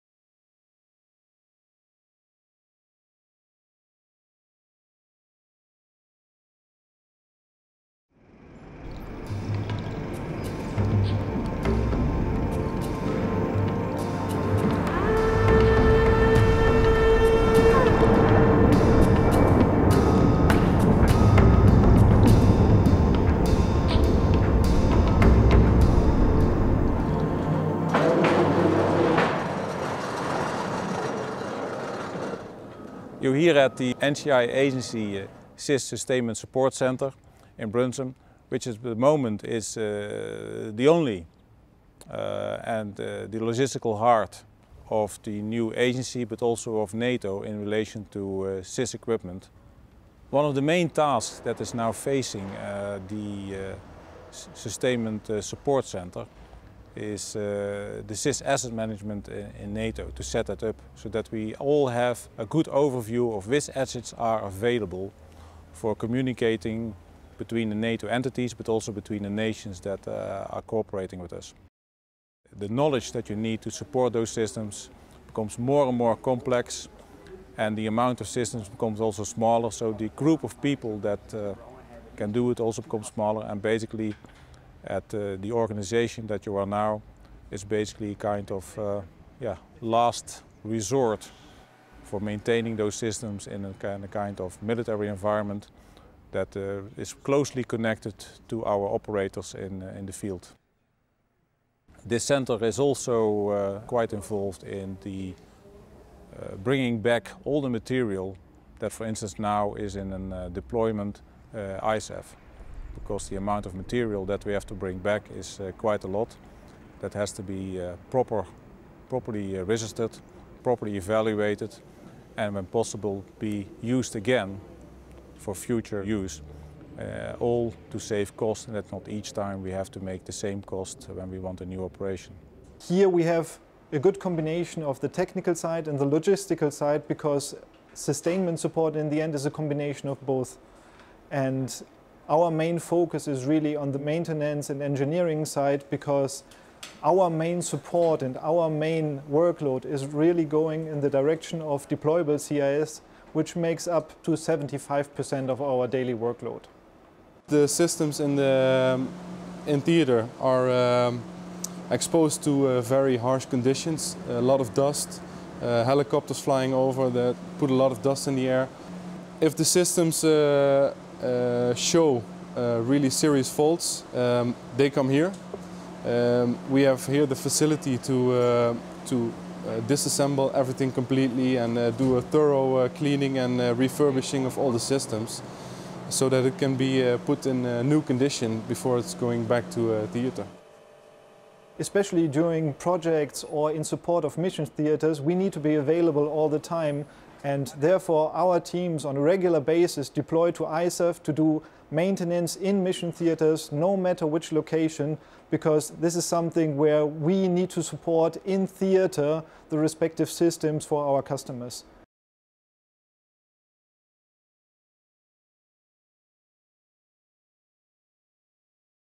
Ensuring operational readiness - CIS Sustainment Support Centre
The NCI Agency CIS Sustainment Support Centre (CSSC) in Brunssum is the NATO sustainment centre - particularly for mobile Communications and Information Systems (CIS) - but also the specialized entity providing engineering and maintenance, and material management to the Agency and its customers.